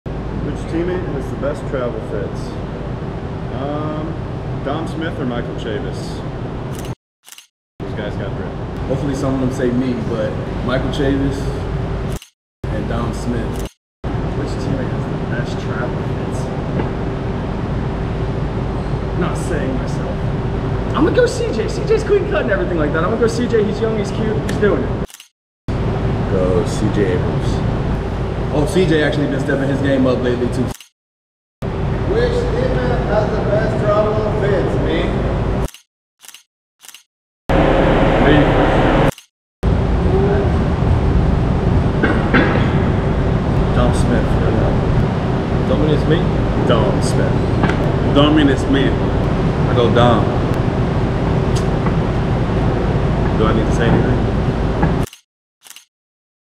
NATS TRAVEL 'FITS: Who Has the Best? 👔
Watch as Washington Nationals Josiah Gray, Keibert Ruiz, Joey Meneses and others discuss which teammates have the best travel attire.

Make your way to Nationals Park to see all your favorite Nats live

Follow us to keep track of everything happening on the field, and off: